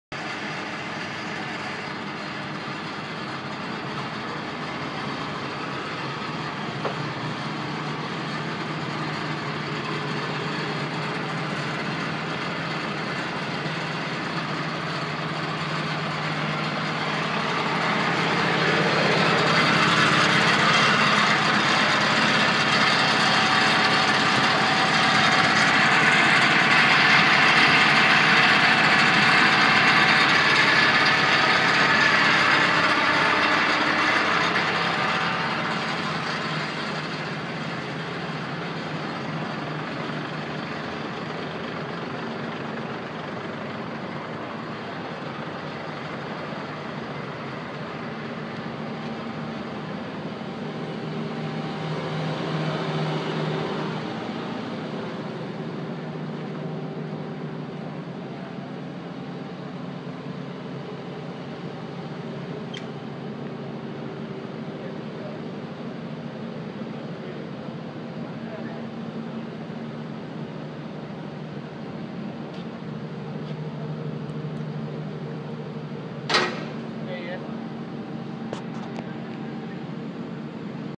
Reed Pump use for Grouting in Singapore MRT Project
Get the brochure here!

Reed Pump in Singapore use for MRT Project as Concrete Pump ,shotcrete Pump and Grouting Pump.